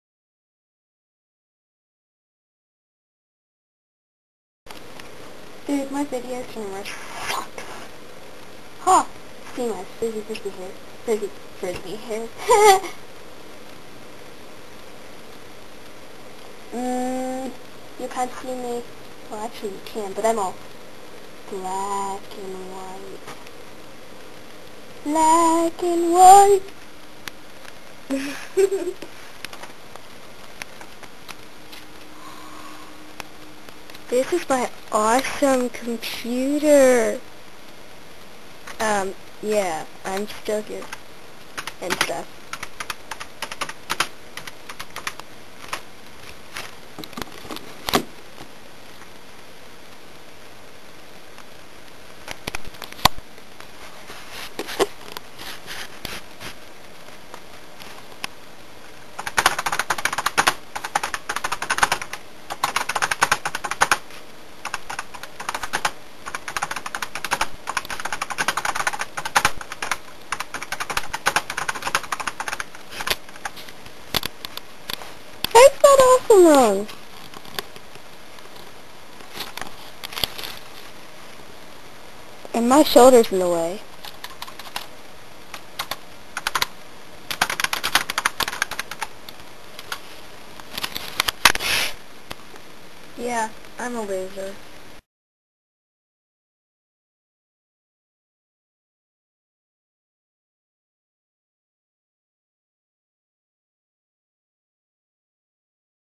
Morning boredom...
This is what happens when you catch me with no sleep. I play with my camera. Fear the mega brightness. The lighting sucks and I was messing around with things.